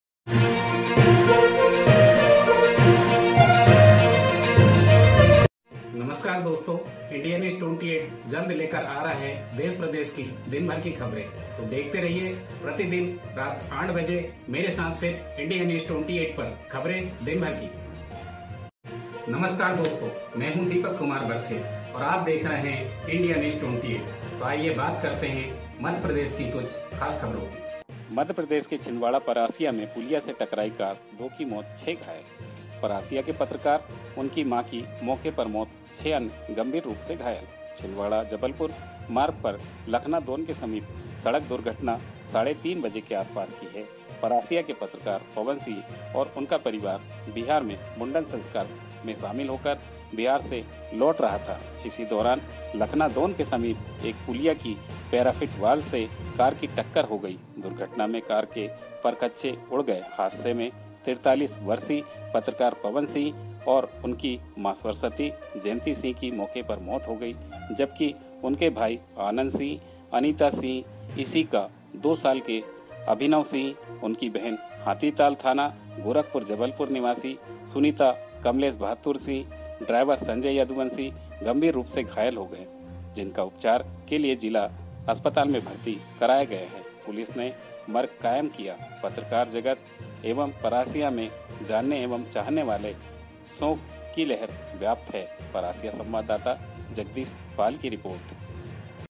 छिंदवाड़ा/परासिया में पुलिया से टकराई कार, दो की मौत 6 घायल
Official